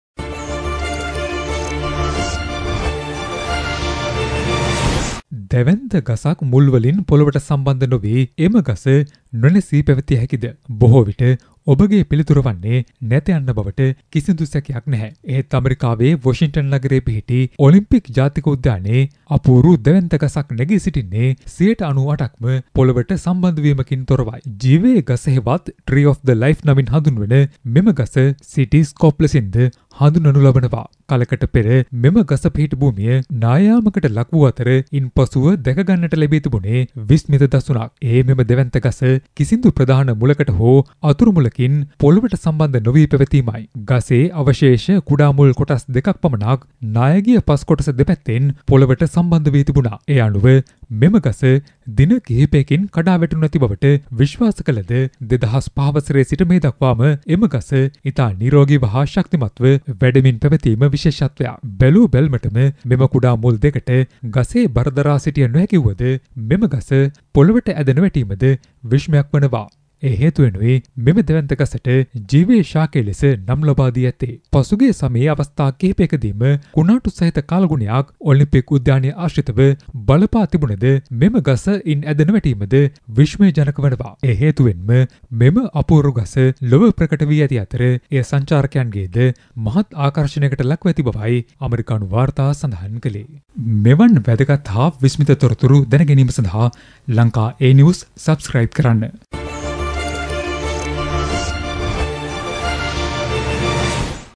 මුල් පොලවේ නැතිව හැදෙන දැවැන්ත ජීවයේ ගස මෙන්න - Tree of Life
දැවැන්ත ගසක් පොලව හා සම්බන්ධ නොවී ශක්තිමත්ව වැඩෙනු ඇතැයි ඔබ විශ්වාස කරනවාද ? එවන් විශ්මිත දසුනක් ලෝකයේ දැකගත හැකි එත් ස්ථානයක් පවතින අතර ඒ අමෙරිකාවේ වොෂින්ටන් හී ඔලිම්පික් ජාතික උද්‍යානයයි. මේ එම විස්මිත දසුනයි.